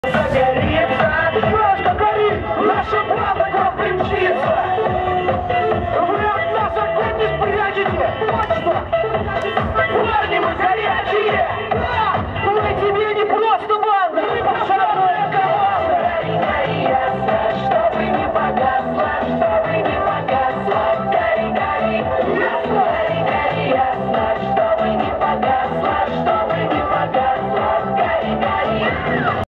Файер-шоу в пожарной части
Файер-шоу на праздновании 50-летия 11-й пожарной части в Зеленограде, 10 ноября 2016 года